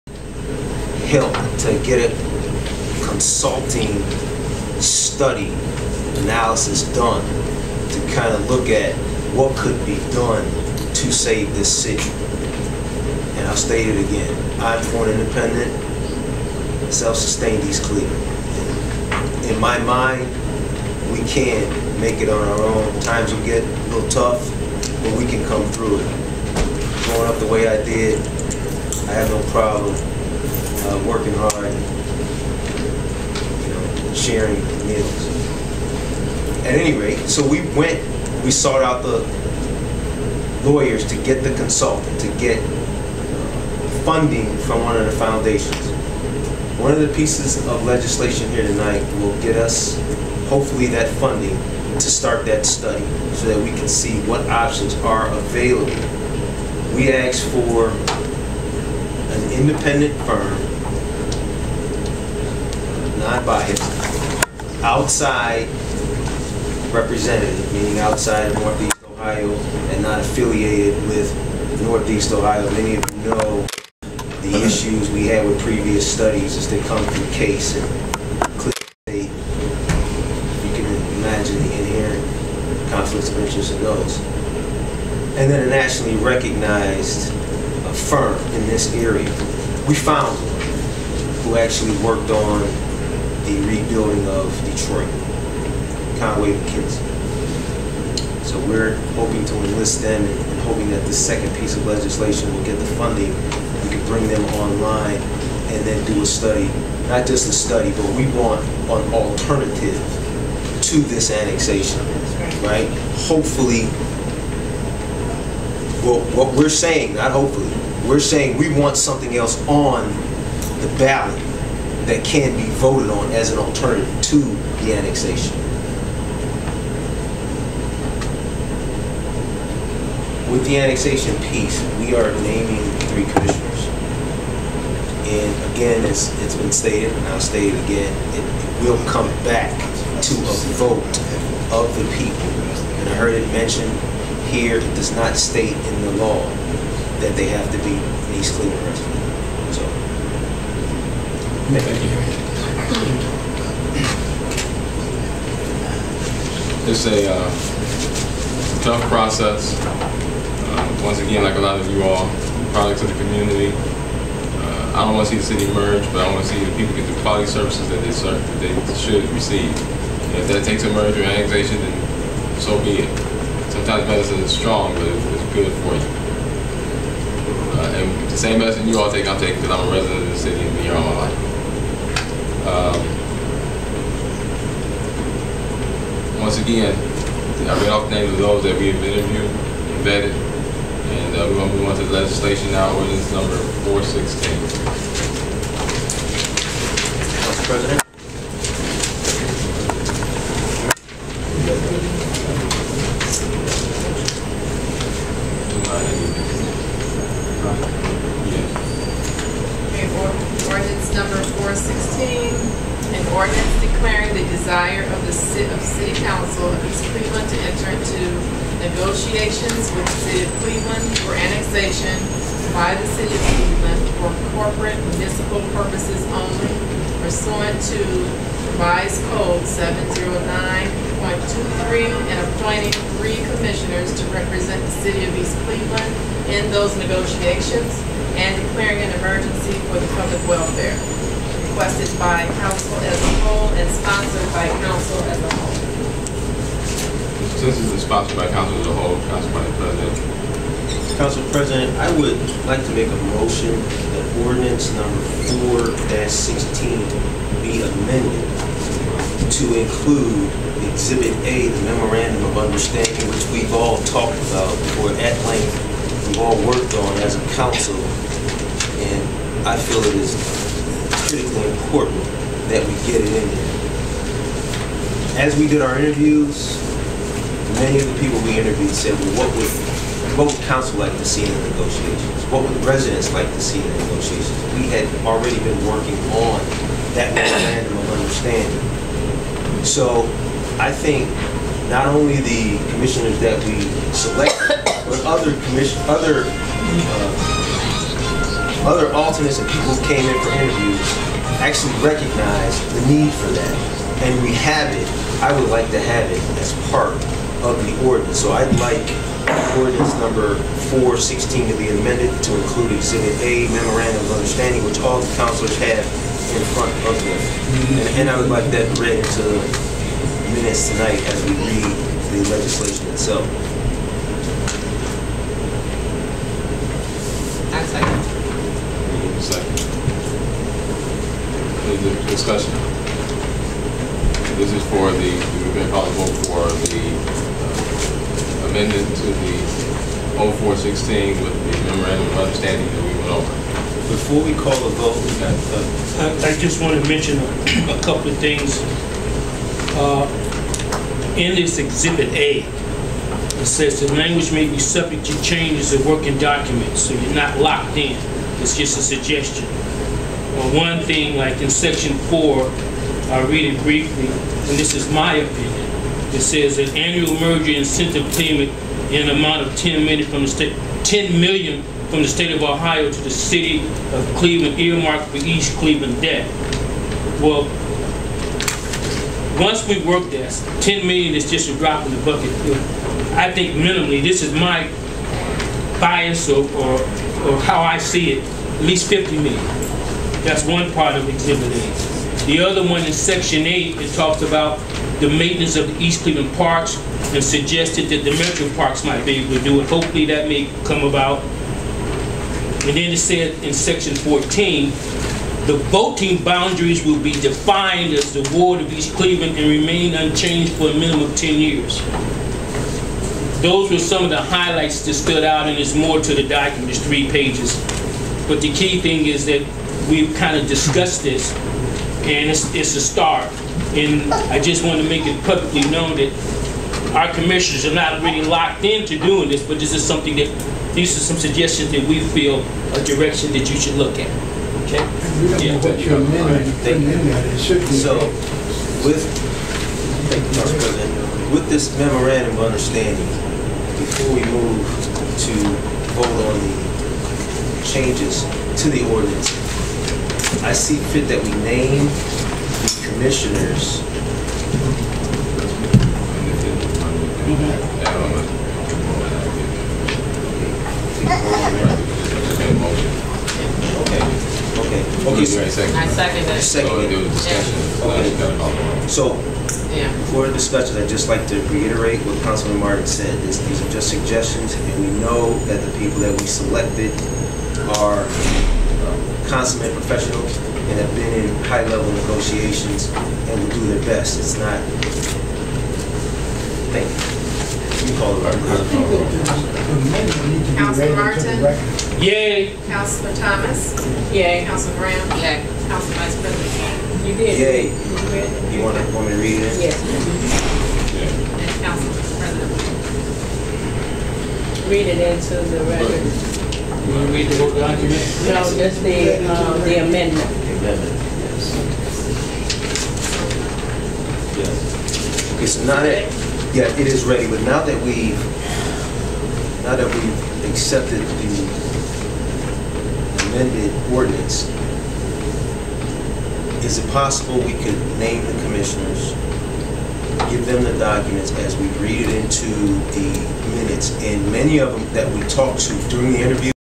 MVI 1377 - Mayor takes over City Council - 8/1/2016 Merger Meeting
East Cleveland Ohio U.S.A. - After being under the control of council members, Mayor Gary Norton Jr. now controls two branches of government.
    The five members of city council were determined to have a secret meeting to pick a group to sell the city of East Cleveland to the mayor and people of sister city Cleveland.
    In this case the public was not allowed to be in on this travesty of justice overlooking state and local laws
    Now somebody has to tell the people of Cleveland that Gary Norton Jr. and Company are trying to break into their city.
    There is no longer a need for public meetings when East Cleveland City Council members as a whole keep things Top Secret.